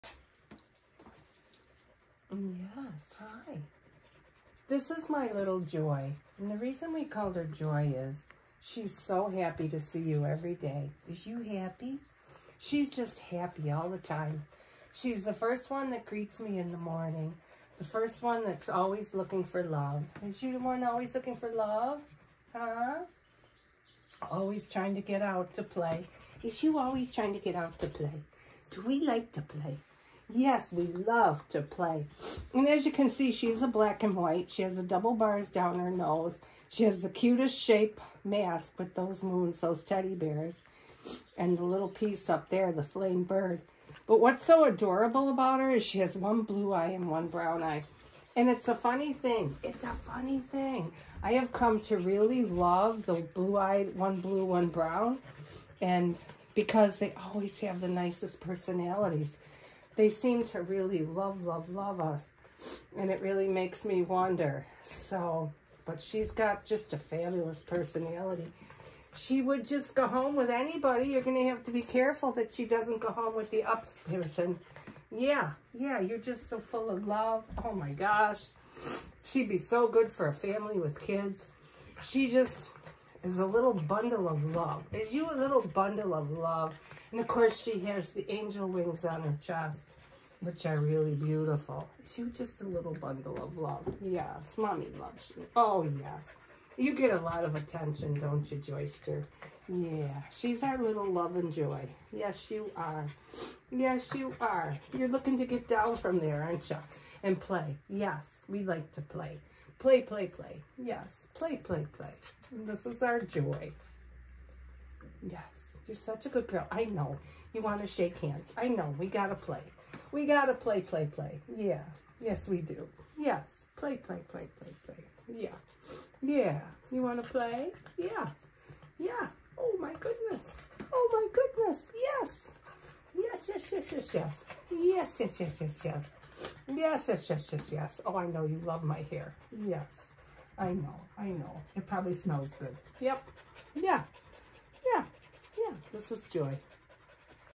Joy our bundle of love and Joy!!!
Joy is the happiest little husky girl. She loves to play and love you. She has one blue eye and one brown eye. I have noticed what a lovely personality the bi-ed puppies have. She has the trinity markings with big teddy bears on her face over her eyes. Double bars down her nose.Just a gorgeous mask.